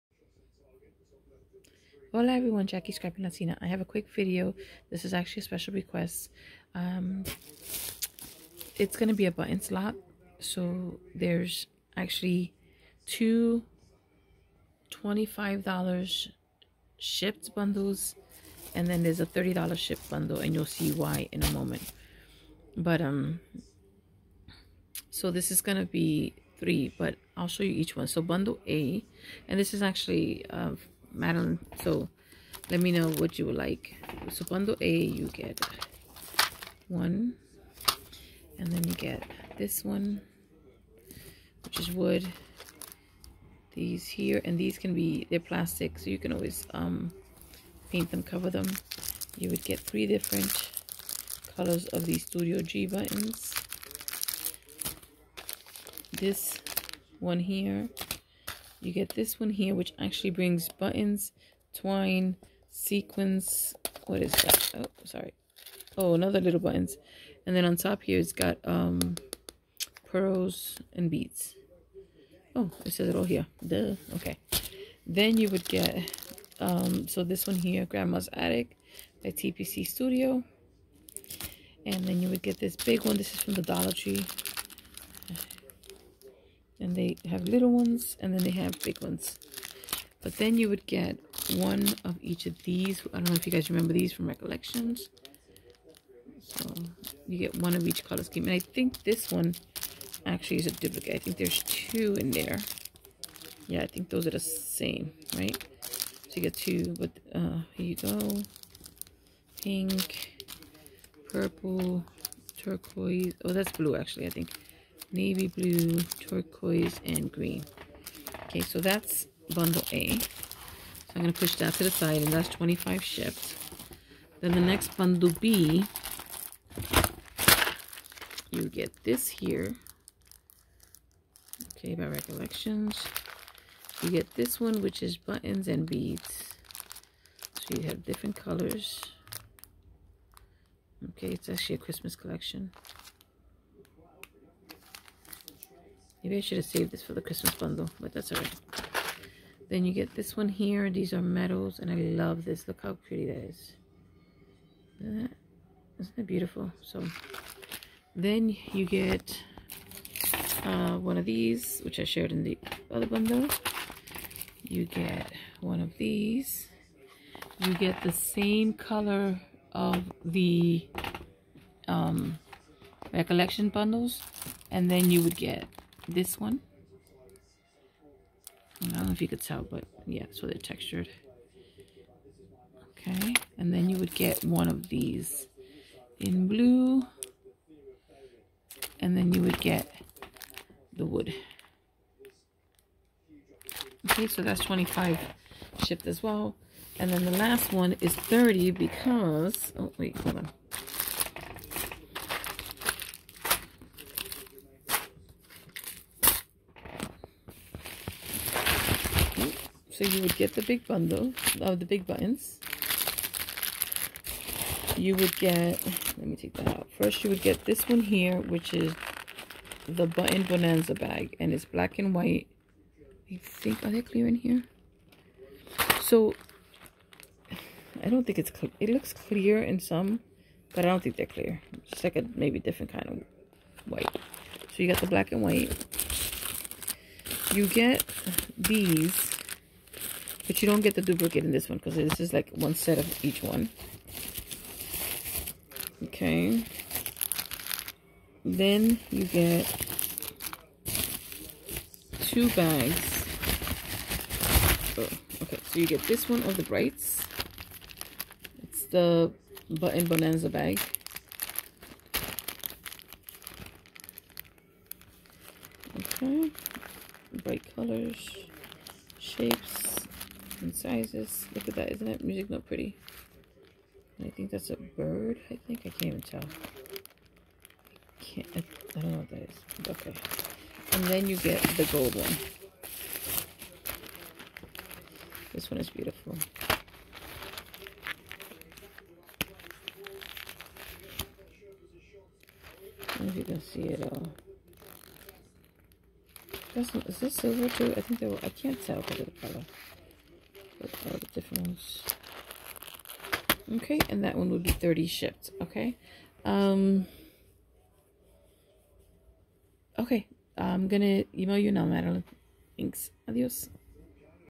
De-Stash: Buttons
Hola everyone.

This is a requested video for a subbie of miscellaneous buttons.

Bundle A = $25.00 shipped
Bundle B = $25.00 shipped
Bundle C = $30.00 shipped - SOLD